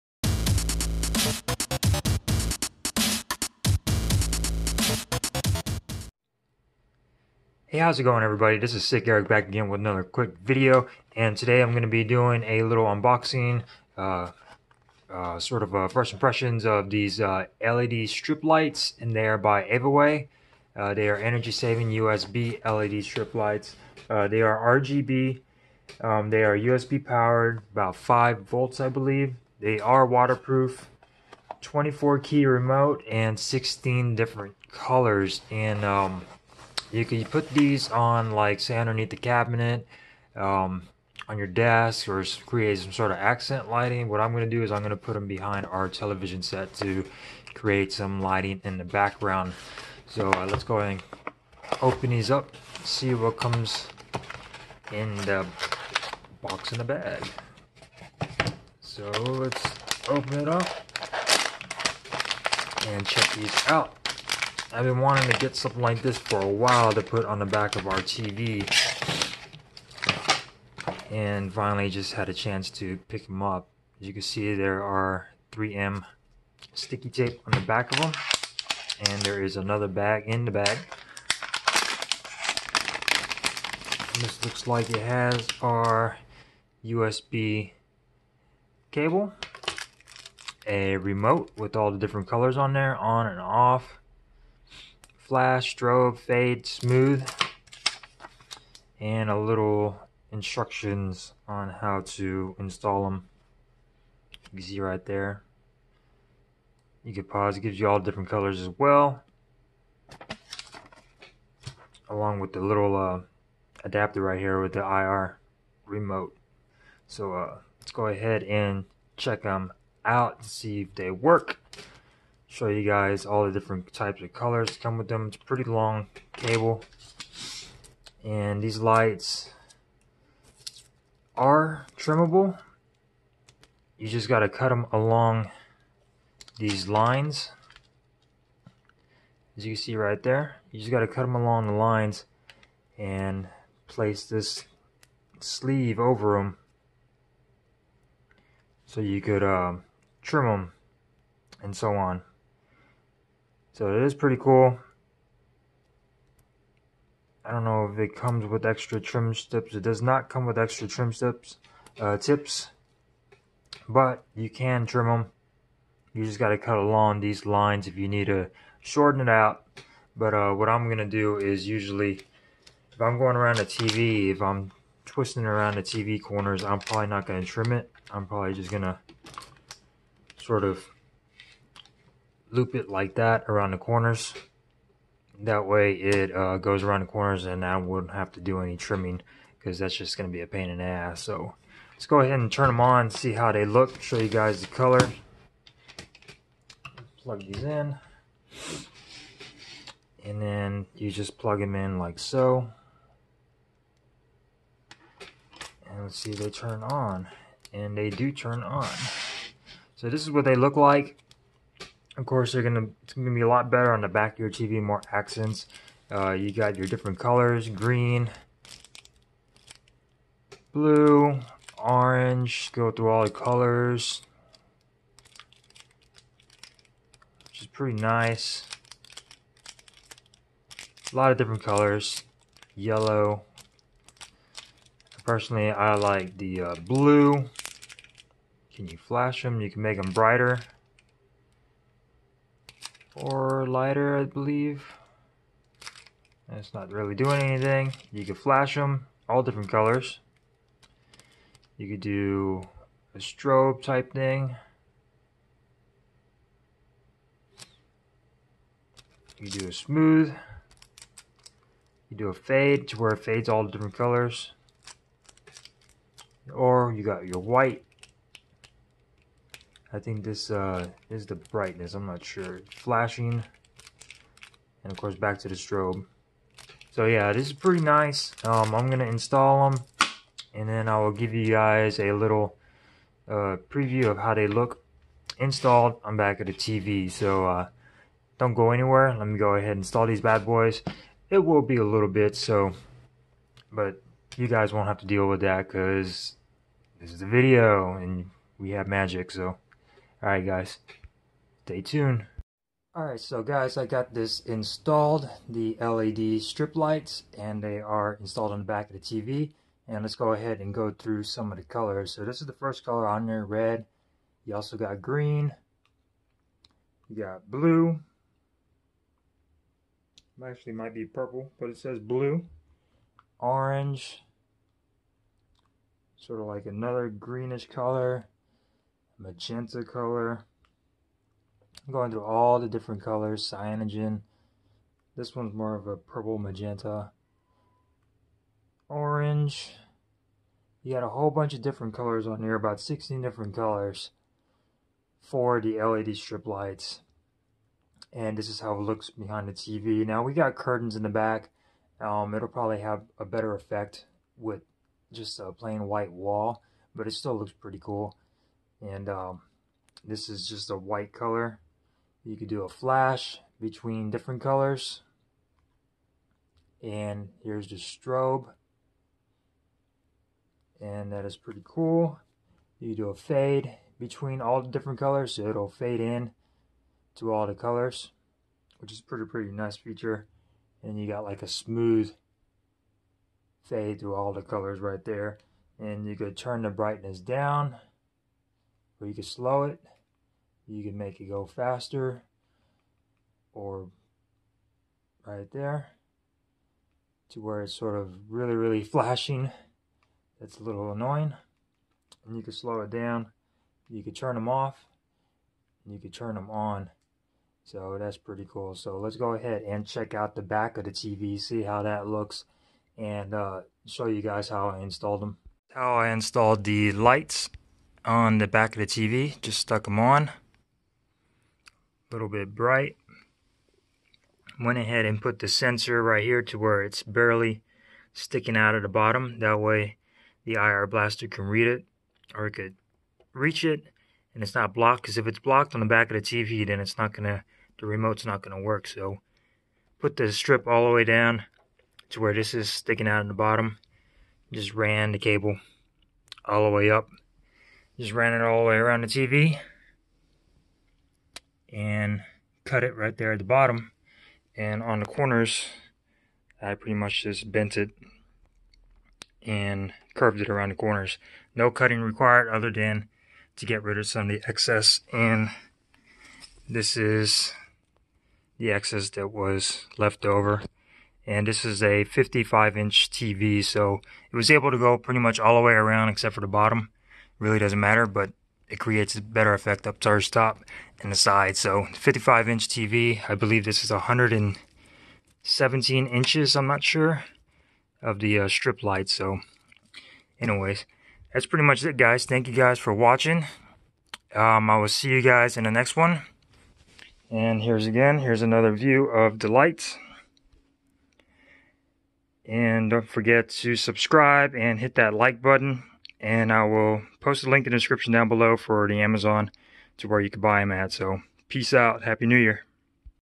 Led Light Strip Unboxing and Review
This is an unboxing video of the USB powered led light strip by AVAWAY. These lights are great for accent lighting and for the back of any flat-screen TV. They are capable of 16 different colors and lighting options and are easy to install.

AVAWAY RGB LED Light Strip, USB Powered 5V SMD 5050 Flexible Waterproof TV Back light with 24 Keys Remote Control for TV Background Lighting PC Notebook Home Decoration - 118 inches/3M